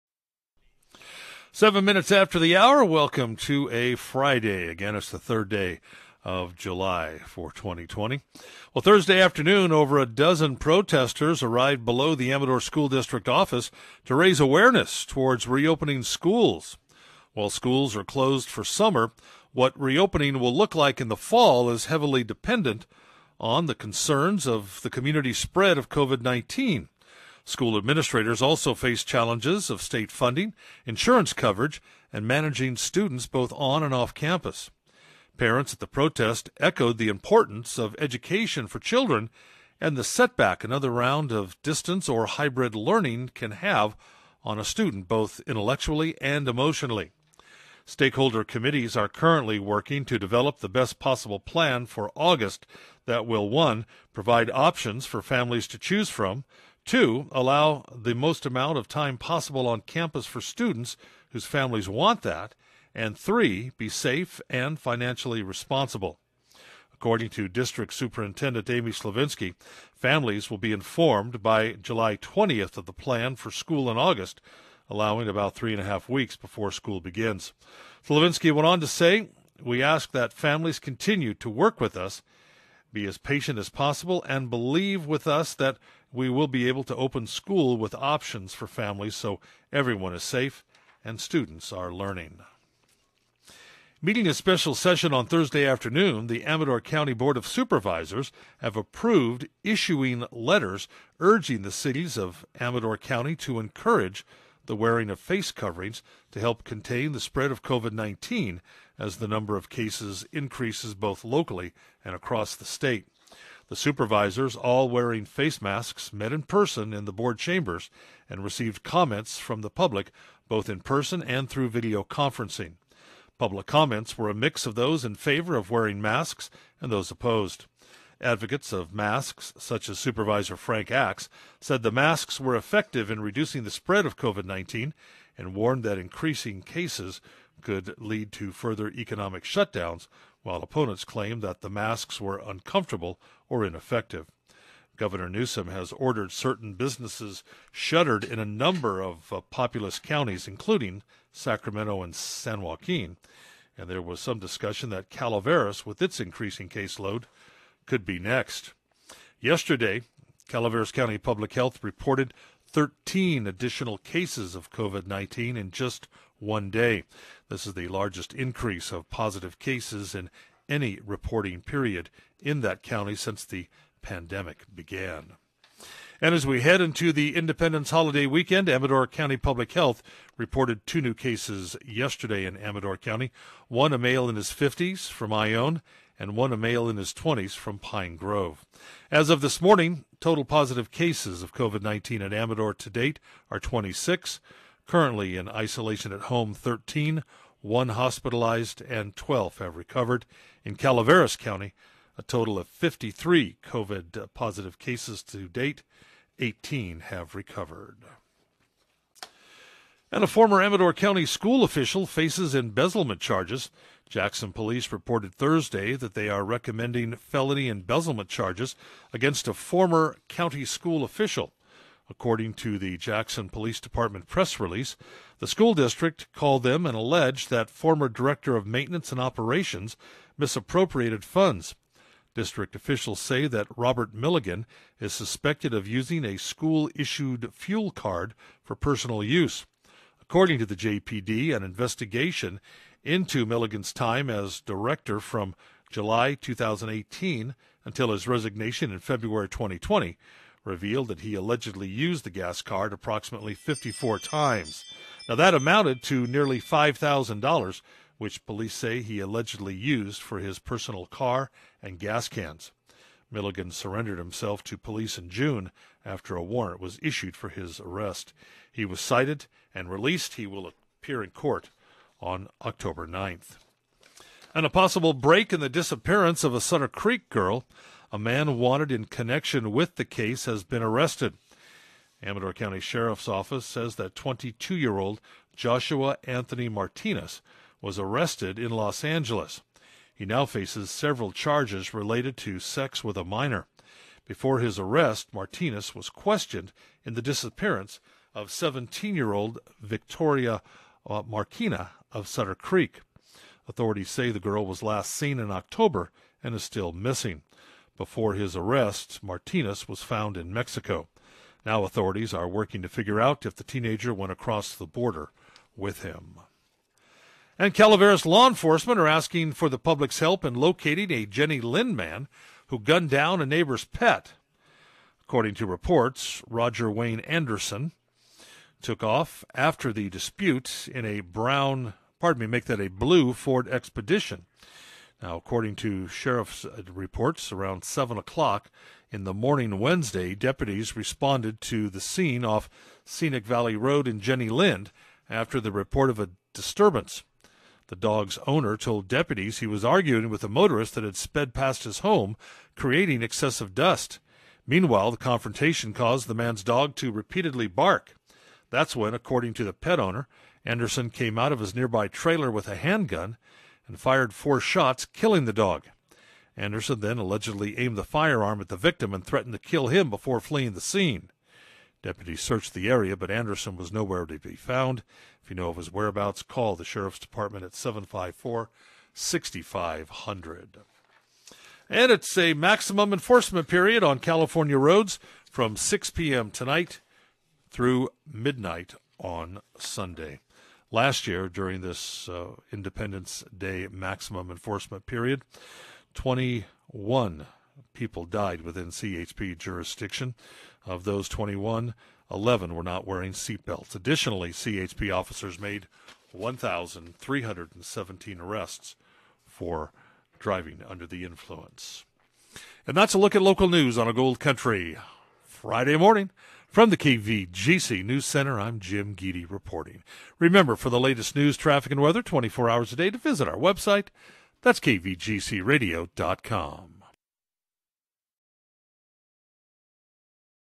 KVGC Local News
Parents Gather To Protest Against The Amador School District.